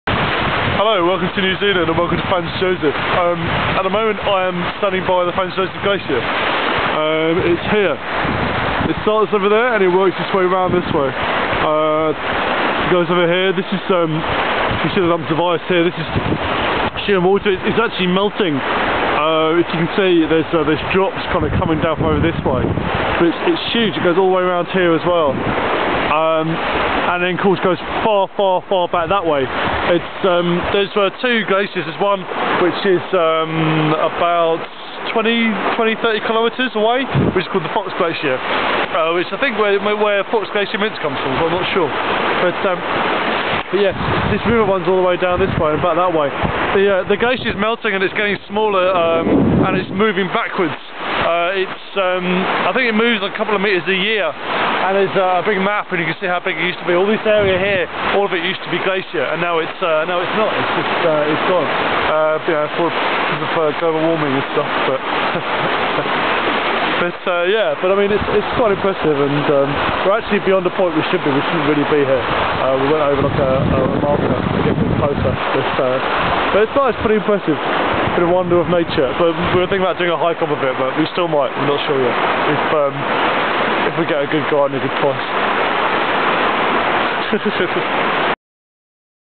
Pete & Patricia Traveling - NZ - Franz Joseph Glacier
This is taken at the Franz Joseph Glacier, showing the glacier and river it creates.